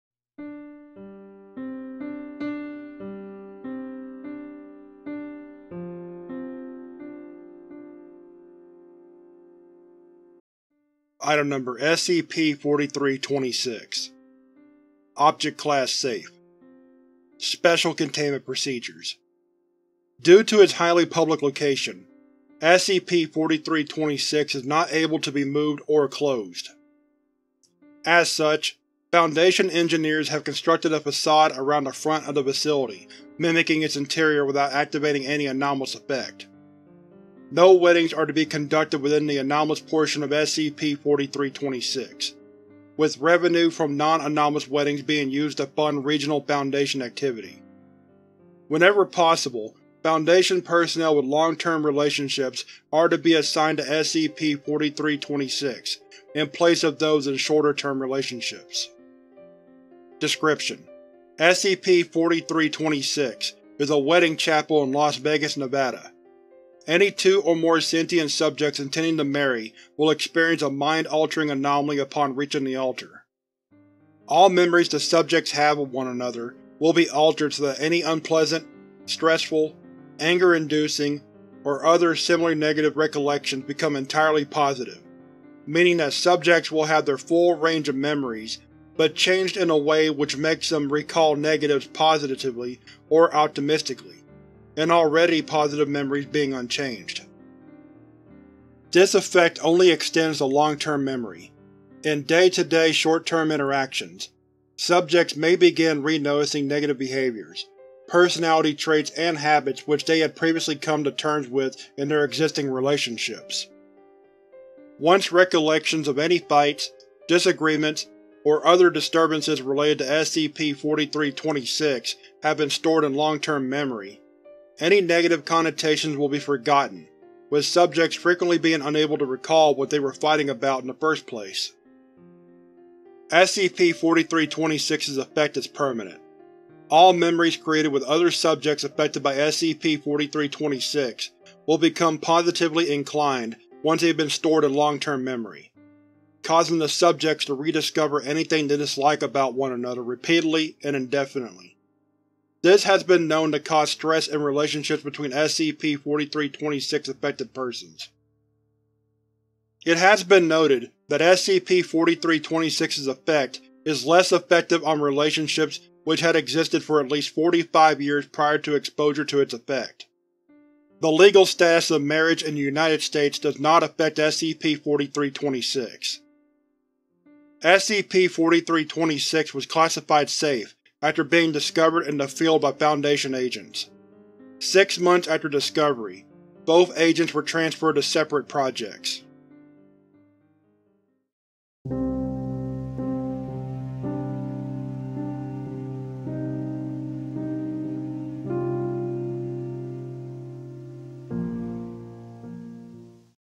The Vegas Chapel That Ruins Relationships (SCP-4326 Honeymooners)
This SCP Foundation reading video is about SCP 4326 "Honeymooners", a wedding chapel in Las Vegas, Nevada that alters wedded people's memories  who get married there, taking any negative memories and forcing them into positive ones, which can cause stress to SCP 4326 affected persons

Table of Contents:
00:00 Introduction
00:59 Description